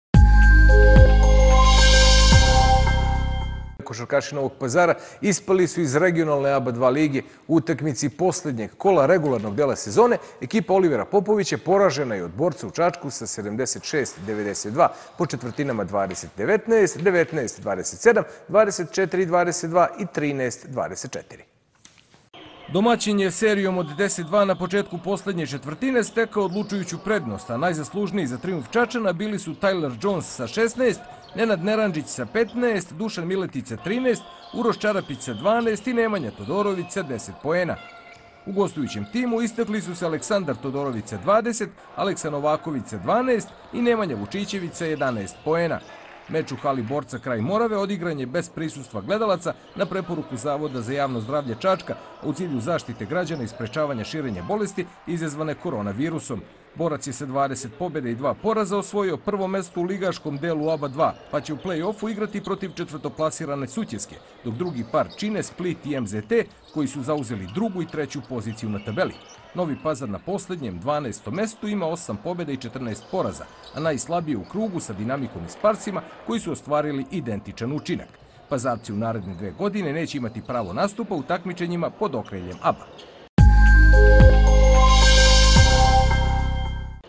Novi Pazar ispao iz ABA 2 lige
Košarkaši Novog Pazara ispali su iz regionalne ABA 2 lige. U utakmici poslednjeg kola regularnog dela sezone, ekipa Olivera Popovića poražena je od Borca u Čačku sa 76:92, po četvrtinama 20:19, 19:27, 24:22 i 13:24.